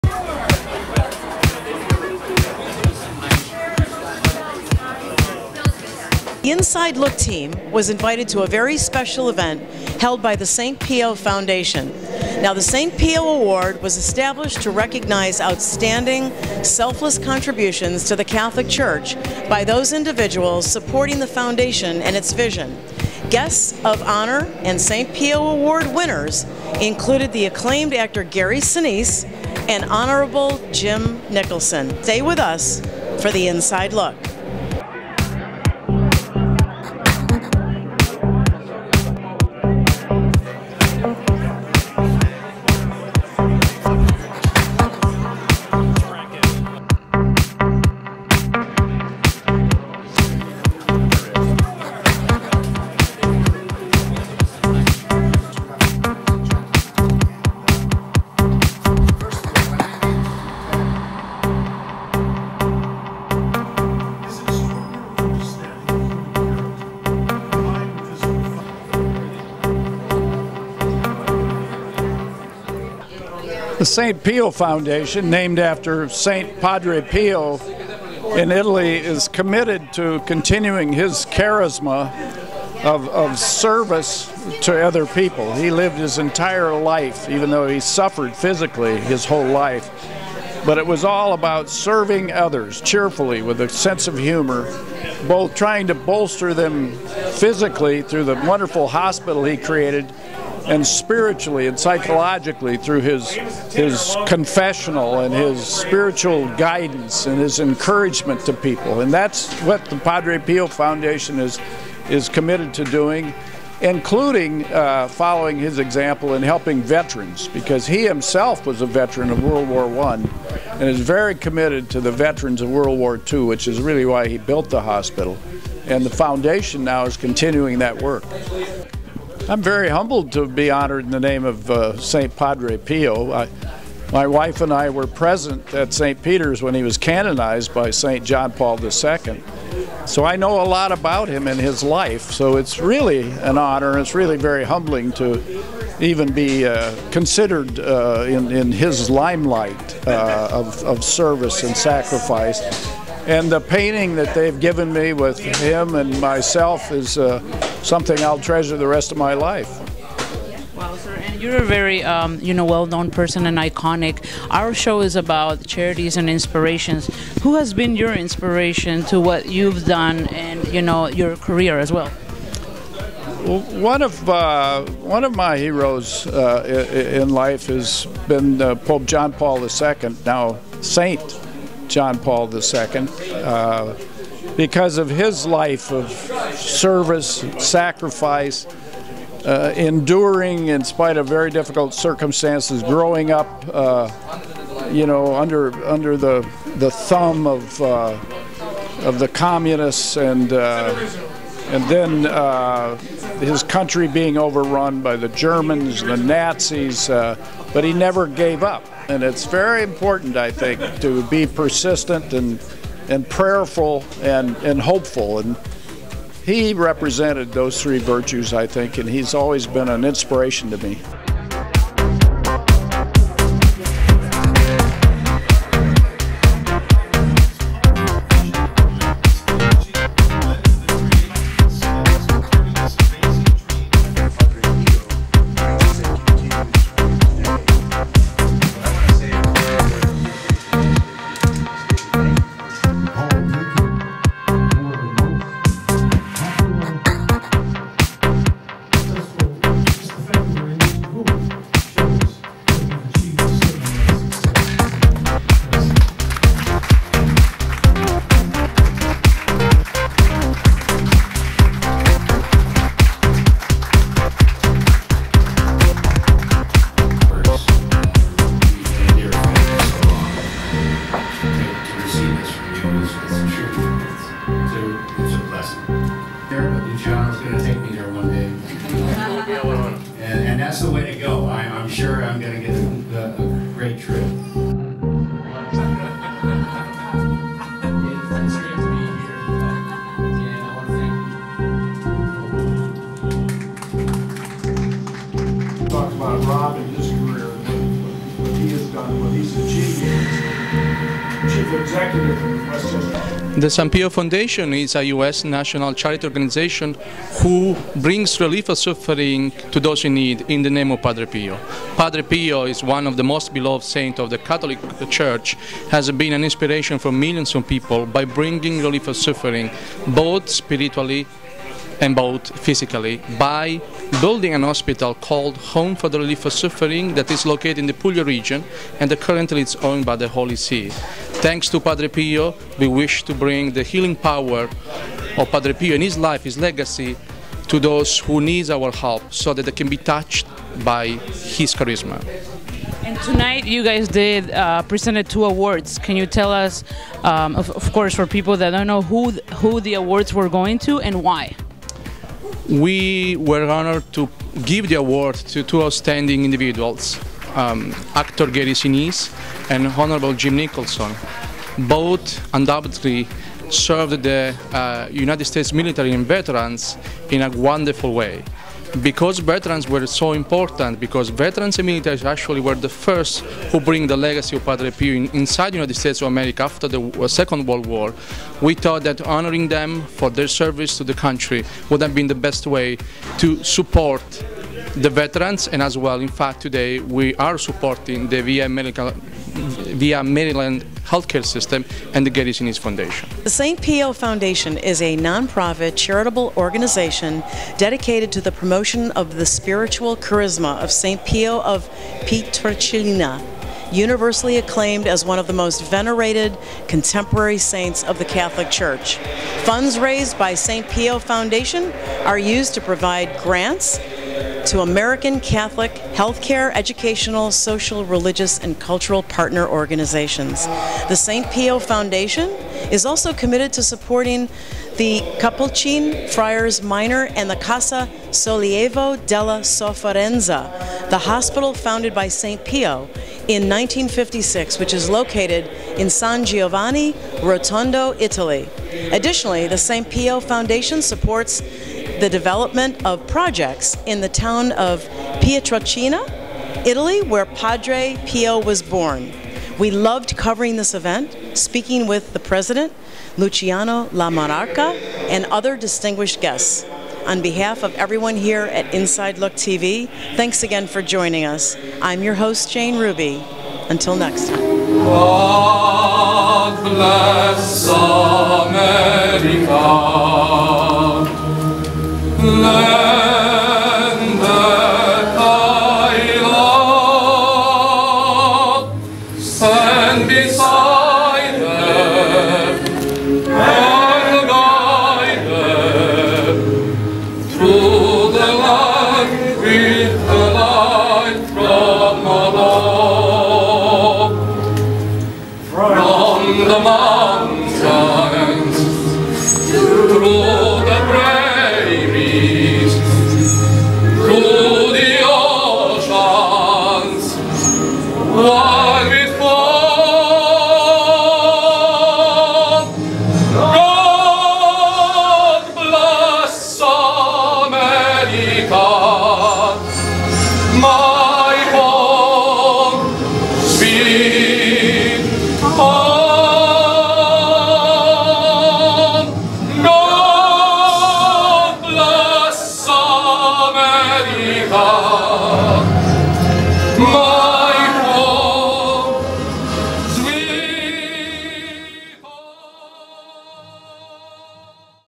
AWARDS CEREMONY WITH GARY SINISE AND JOE MANTEGNA
INSIDE LOOK TV MAGAZINE

PRESENTS

SECOND ANNUAL
SAINT PIO AWARD CEREMONY
WASHINGTON, DC

INTERVIEWS:

HON. JIM NICHOLSON
AWARD RECIPIENT
FORMER SECRETARY FOR
VETERANS AFFAIRS

LUCIANO LAMONARCA
PRESIDENT
SAINT PIO FOUNDATION
TENOR

AN AWARD WAS PRESENTED TO
ACTOR GARY SINISE

JANE RUBY
HOST

GEDALIA VERA
INTERVIEWS

GEDALIA VERA
PRODUCER/DIRECTOR

GEDALiA VERA
SCRIPT

DJ NEEKOLA
MUSIC

LUCIANO LAMONARCA
CREDITS MUSIC

INSIDE LOOK TV MAGAZINE IS EVERYWHERE!

COPYRIGHT OF INSIDE LOOK TV MAGAZINE.